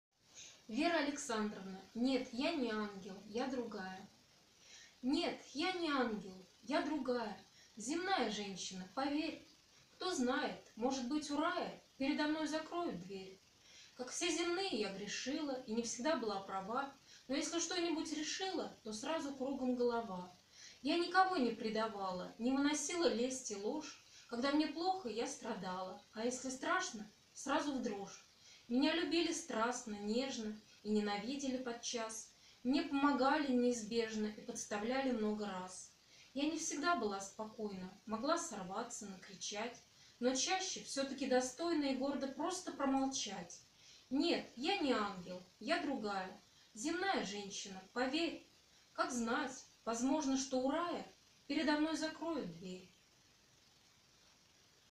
Вера Александровна. "Нет, я не ангел! Я другая." 2008 г.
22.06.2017 г. Читаю стихотворение. Вера Александровна. "Нет, я не ангел! Я другая." 2008 г.